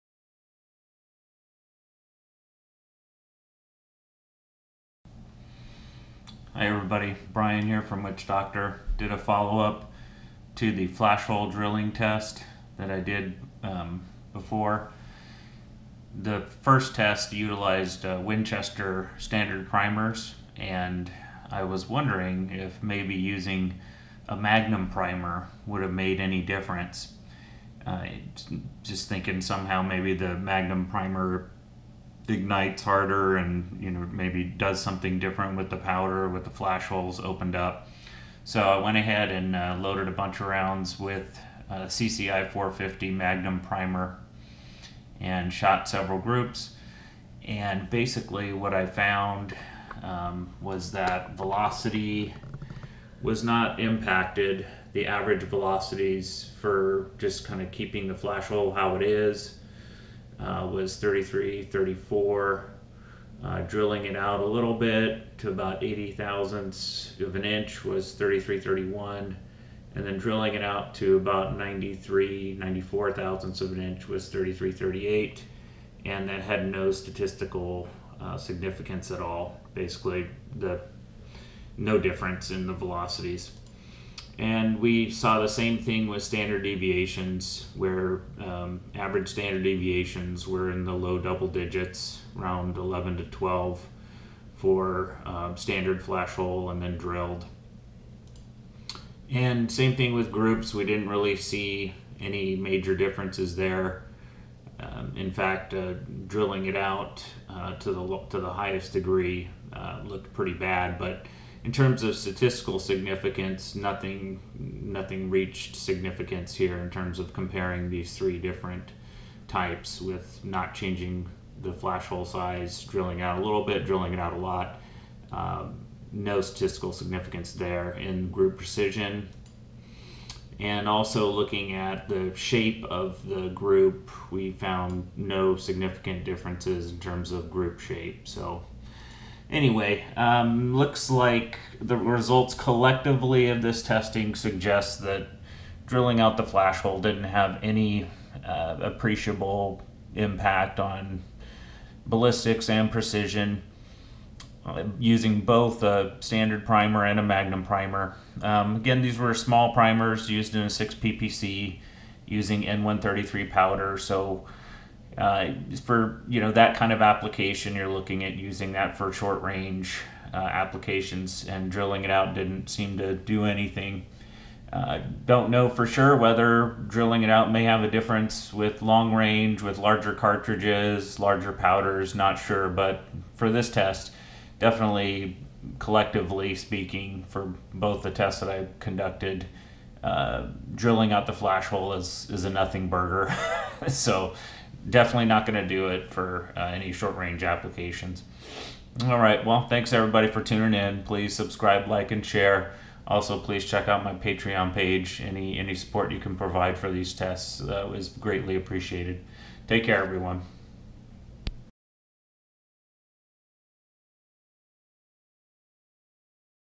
Flash Hole Diameter Follow Up Test with Magnum Primers
The purpose of this video is to describe data regarding flash hole size variations using magnum primers in 6PPC. Results suggest no benefit to ballistics and precision when drilling flash holes to larger diameters.